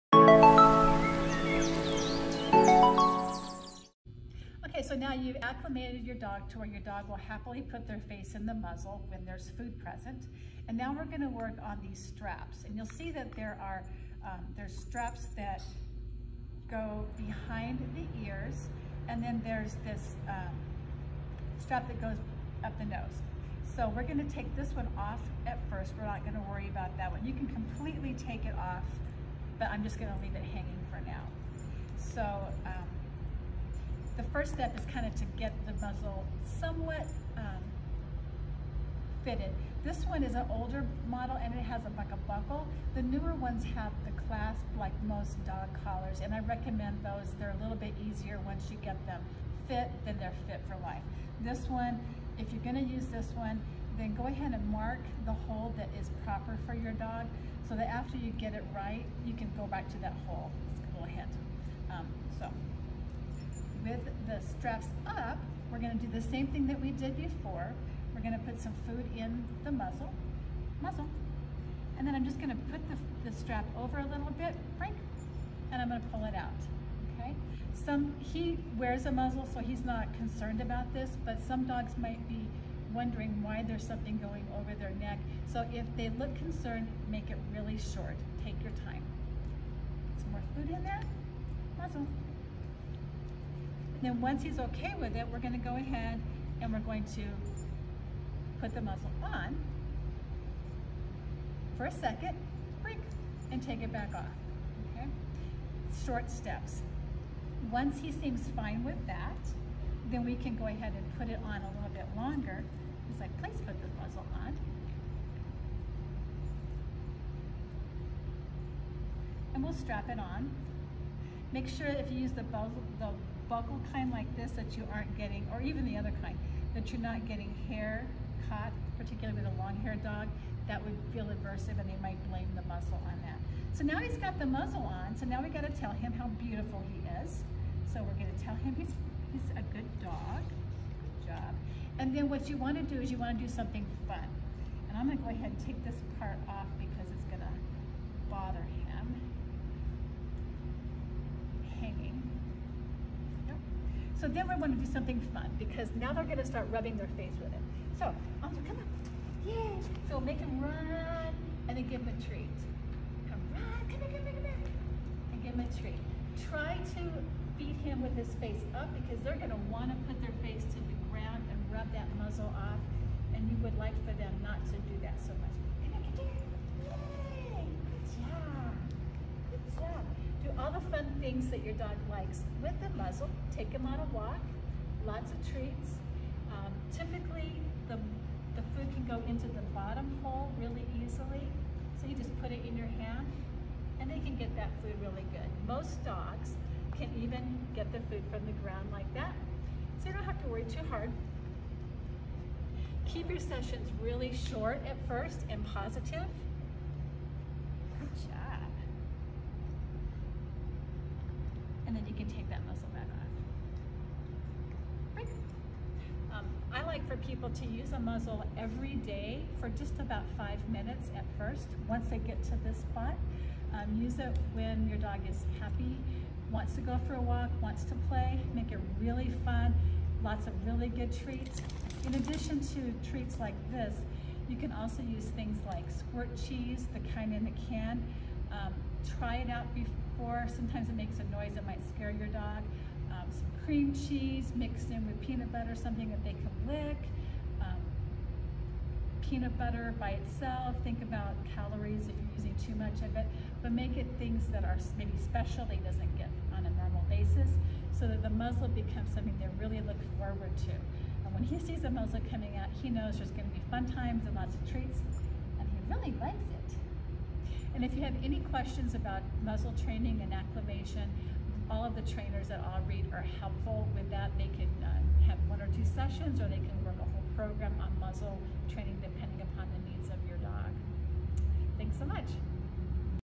All Breed Rescue & Training - Muzzle Training Pt 2
Check out the video for part 2 of how to muzzle train your dog! Thanks Trainer Terje and her furry assistant for showing us how! As we mentioned last week, muzzle training is a super important and handy thing to teach your dog! And you can make it a fun and rewarding experience for them!